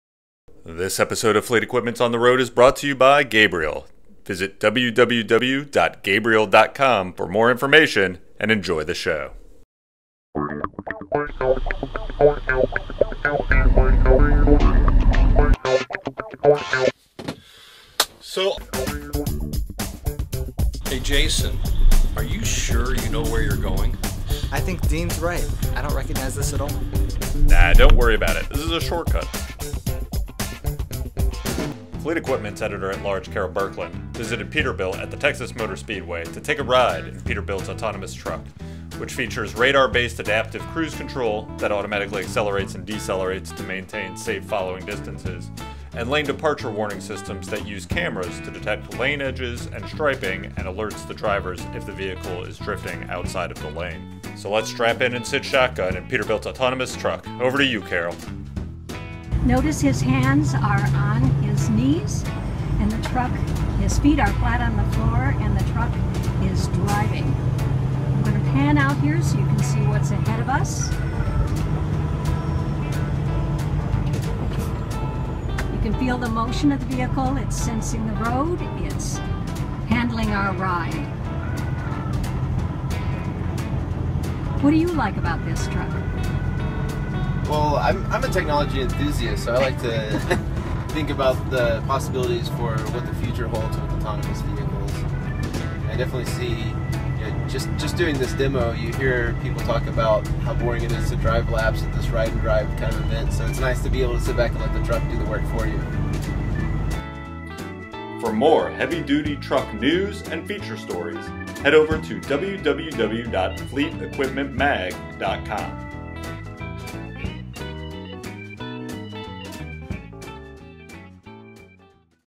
Peterbilt Autonomous Truck Ride-Along
Fleet Equipment Editor-At-Large Carol Birkland visited Peterbilt at the Texas Motor, to take a ride in Peterbilt’s autonomous truck, which features Radar-based adaptive cruise control (ACC) that automatically accelerates and decelerates to maintain safe following distances and Lane departure warning systems that use cameras to detect lane edges and striping and alerts the driver if the vehicle is drifting outside of its lane.